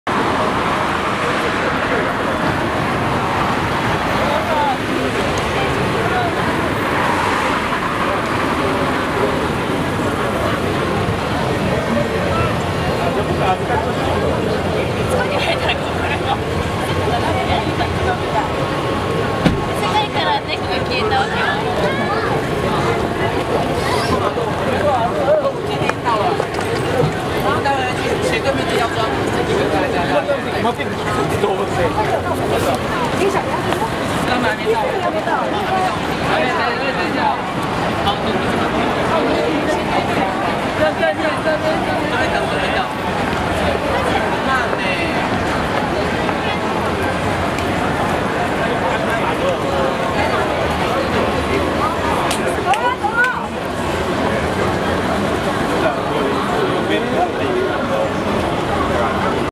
Shinjuku Tokyo's Yasukuni Dori Night Time Lapse
1500 1/4 second shutter speed night time lapse of Yasukuni Dori in Shinjuku, Tokyo Japan.